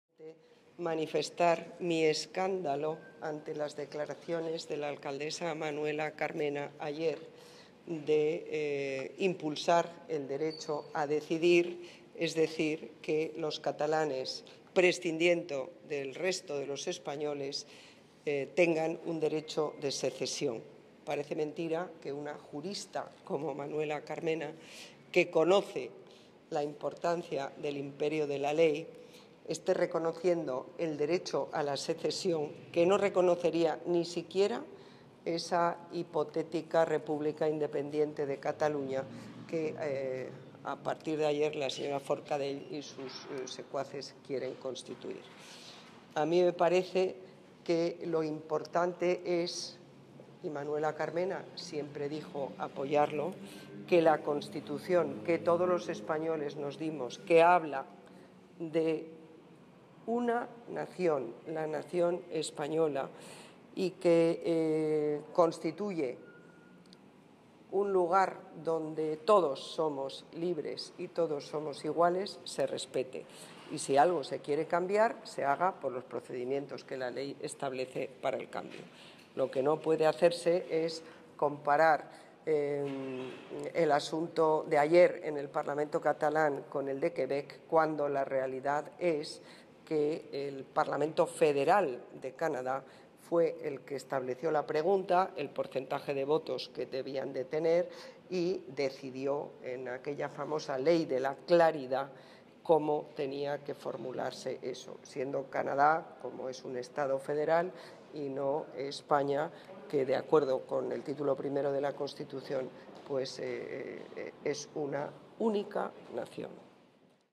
Las declaraciones de Carmena sobre Cataluña son un escándalo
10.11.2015

Quiero manifestar mi escándalo ante las declaraciones que hizo ayer la Alcaldesa de impulsar el derecho a decidir, es decir, que los catalanes, prescindiendo del resto de los españoles, tengan un derecho a la secesión.

Parece mentira que una jurista como Manuela Carmena, que conoce la importancia del imperio de la ley, esté reconociendo el derecho a la secesión. Un derecho que no reconocería ni siquiera esa hipotética república independiente de Cataluña que quieren construir Forcadell y sus secuaces.

Creo que lo importante es que la Constitución que nos dimos todos los españoles, que habla de una única Nación que constituye un lugar donde todos somos libres e iguales, se respete. Y si algo se quiere cambiar se haga por los procedimientos que establece la ley.